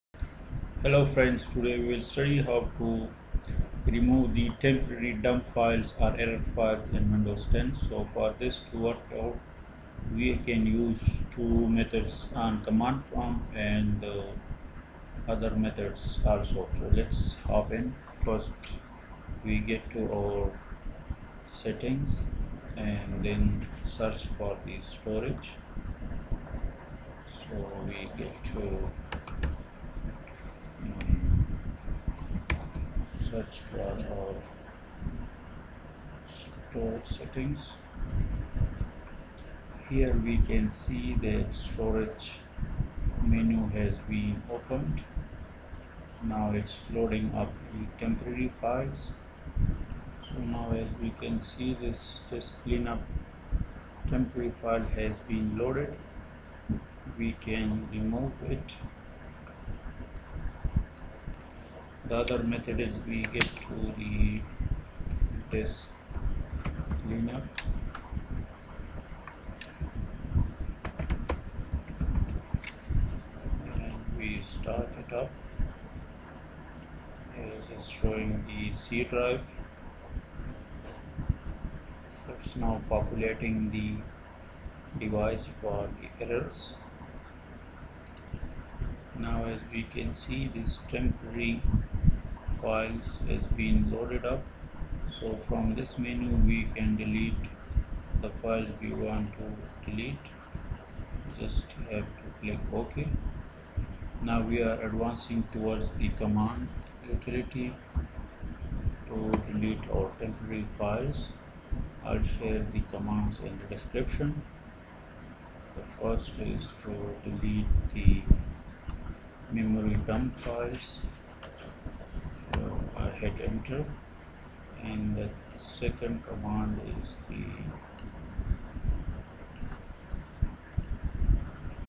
How to Remove System Error Memory Dump Files on Windows 10
del /f /s /q %systemroot%\memory.dmp

del /f /s /q %systemroot%\minidump\*.*